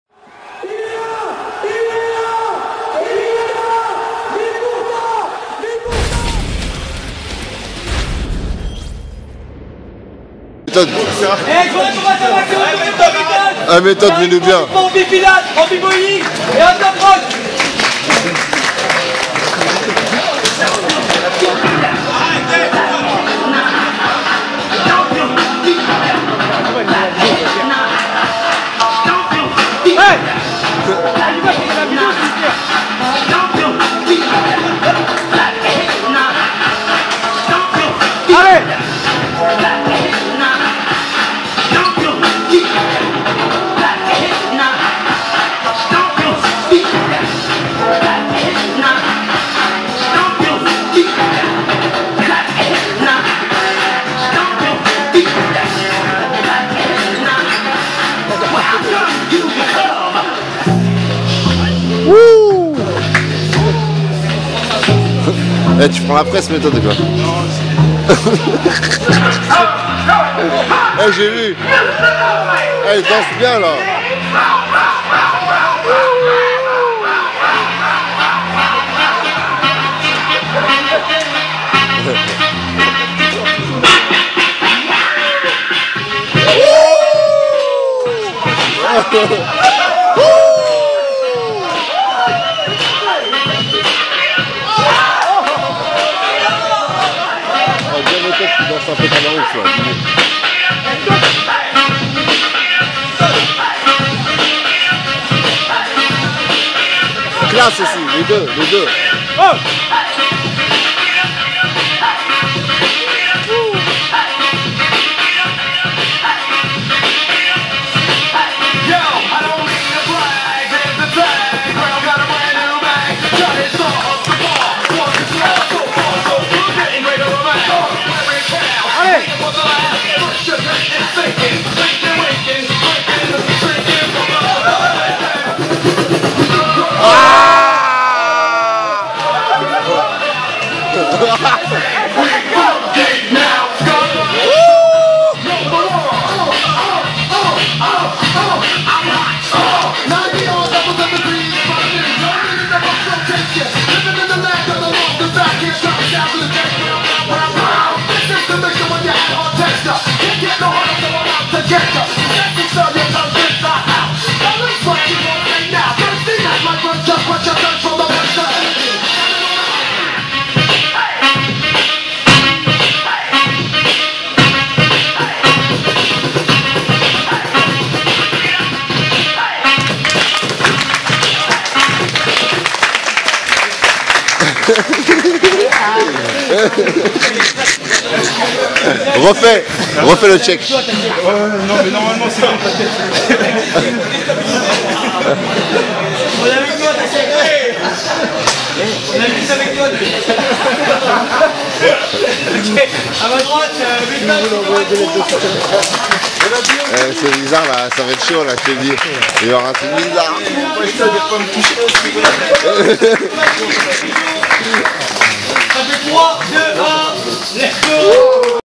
" SUPER B.BOYS" - METHOD VS LEEZA by YOUVAL
" SUPER BBOYS " ( contest Underground B.boyin' et Top rockin')
Organiser par YOUVAL
Jurys: KARIM BAROUCHE-DOM K-KAPELA
Dj: Senka
Speakers: MAGIK YOUVAL